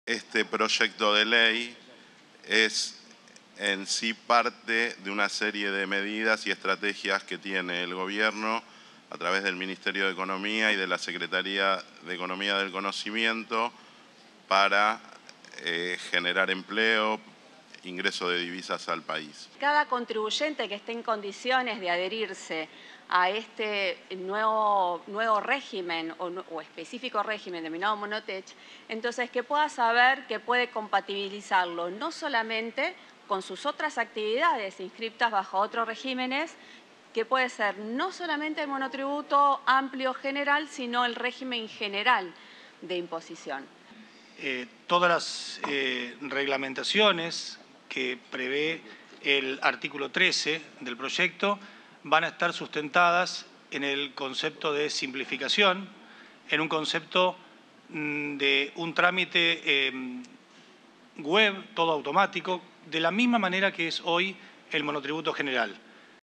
CON ANÁLISIS DE FUNCIONARIOS, COMENZÓ EN DIPUTADOS DEBATE SOBRE PROYECTO DE MONOTRIBUTO TECNOLÓGICO
CON EL ANÁLISIS DE FUNCIONARIOS, COMENZÓ EN DIPUTADOS EL DEBATE SOBRE EL PROYECTO DE MONOTRIBUTO TECNOLÓGICO